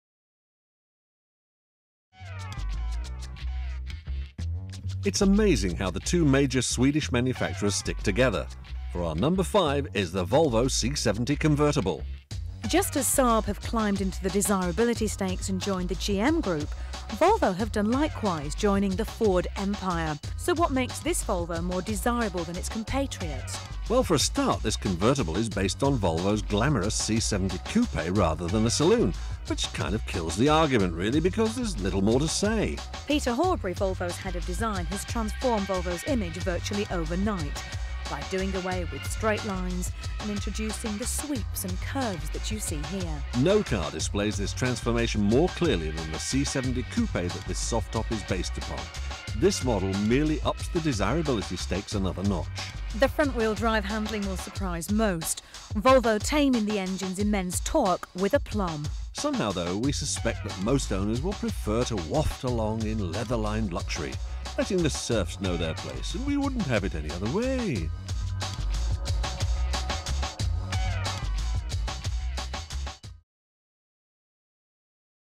2001 Volvo C70 Convertible Overview
Ginny Buckley and Dave Lee Travis take a look at the 2001 model Volvo C70 Convertible and give us an overview of some of it's features, including it's transformation of it's image, upping he desirability stakes. This car is placed at number five on the list of their top 10 convertibles.